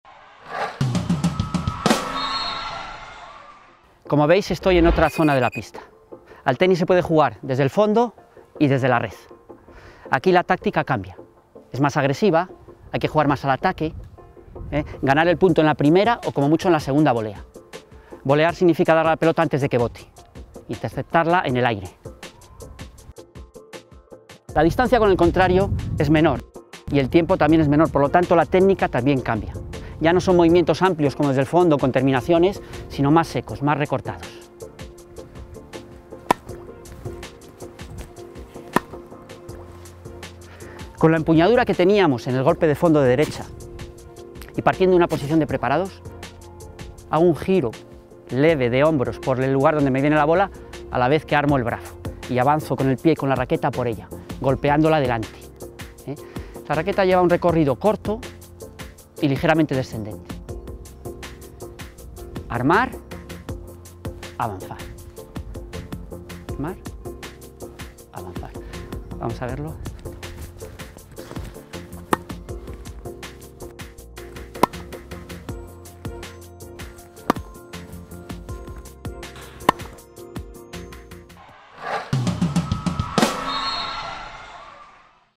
Tenis 6. Bolea de derecha
Antonio de Anta, director de la escuela de tenis de Vega Sport, nos enseña a realizar la bolea de derecha.
Muy pronto nuevos vídeos.